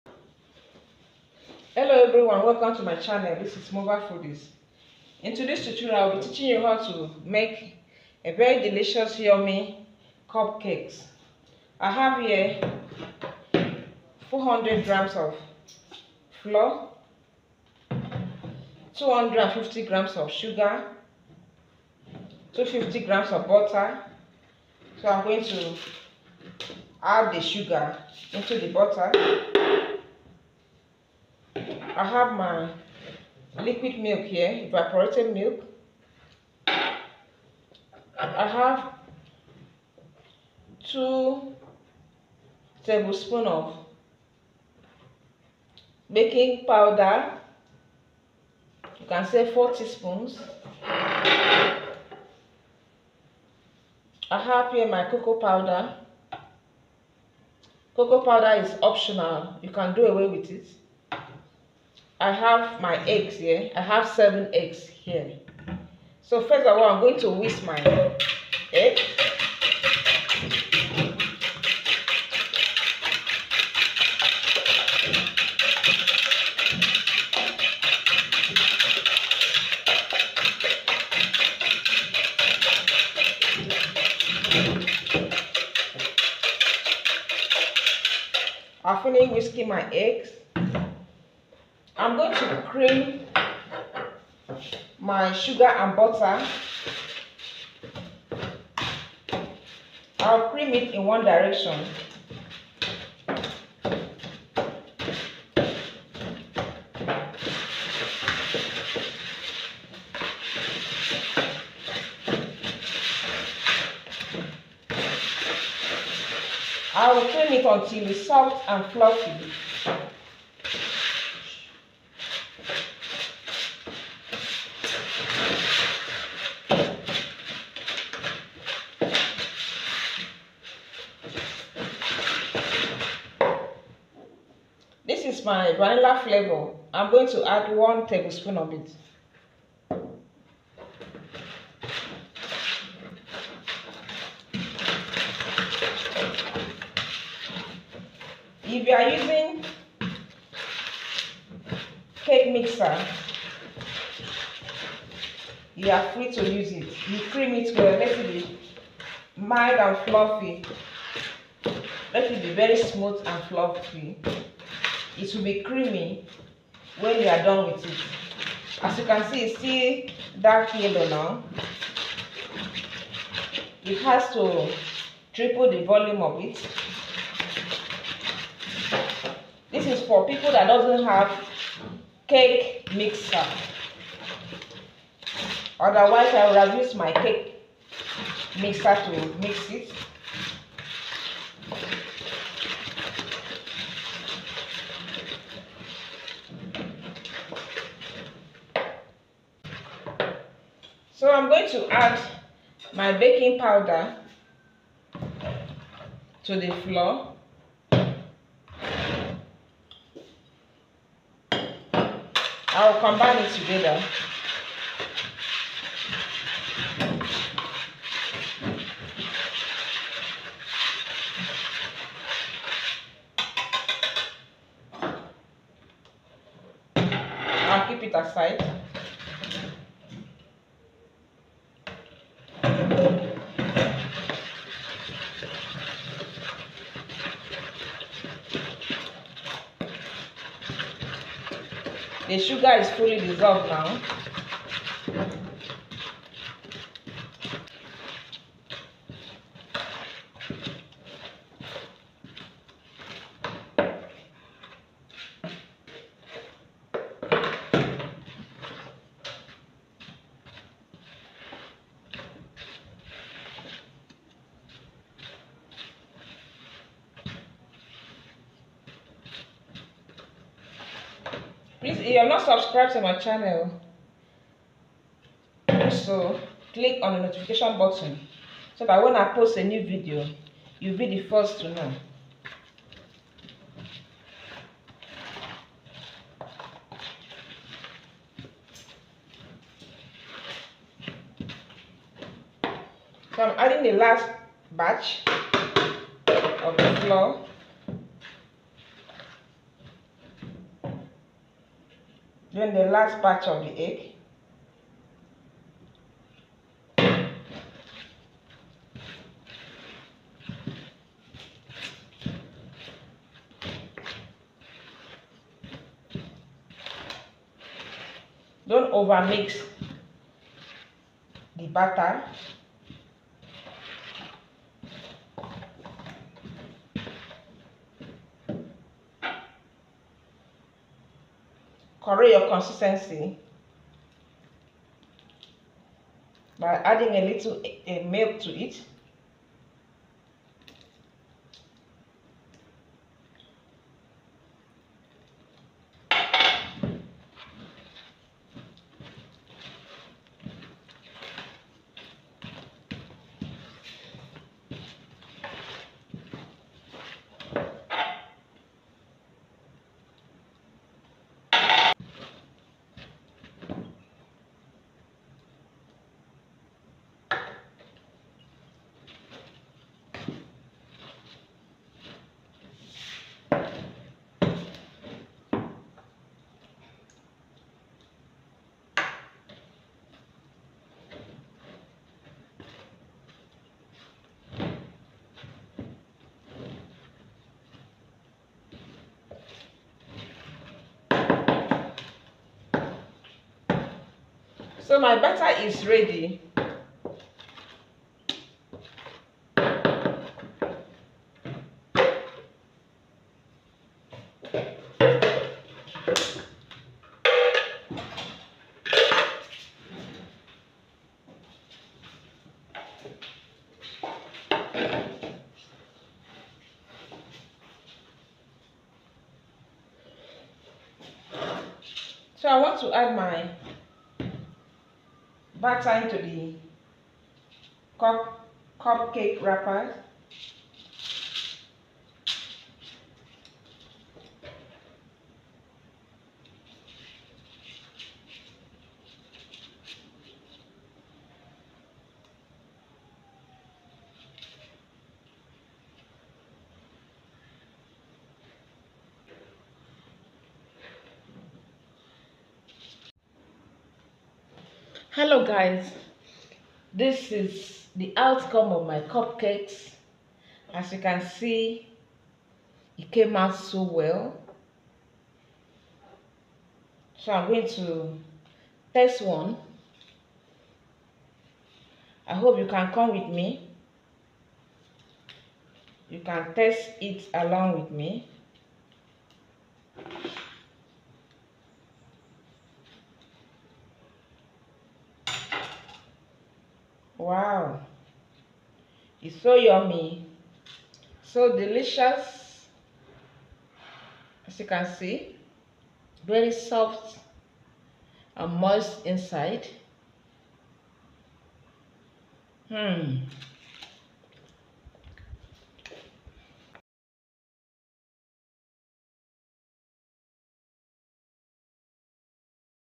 MAKE THIS DELICIOUS PARTY CUPCAKES FOR CHILDREN.
Make this delicious party cupcakes for children. They will like it and thank you.
Recipe in the video.